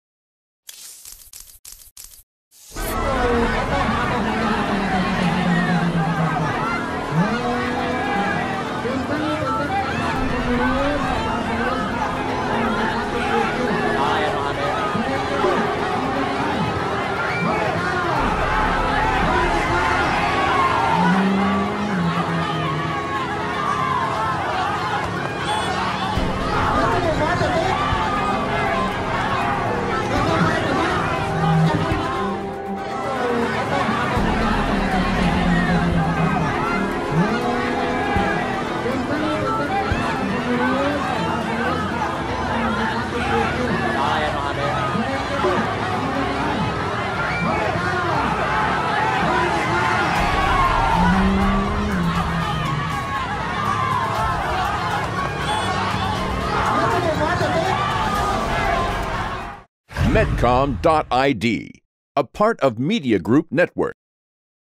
Pawai Peringatan HUT ke-77 RI di Garut Berakhir Tawuran
Sekelompok massa di Garut, Jawa Barat pada Rabu (17/8) siang kemarin tawuran saat pawai peringatan HUT RI ke-77.

Massa yang didominasi remaja itu tawuran ketika arak-arakan pawai menuju sebuah lapang. Diduga tawuran dipicu adanya salah paham saat penggunaan lodong yang merupakan permainan tradisional yang biasa digunakan untuk memeriahkan HUT Kemerdekaan.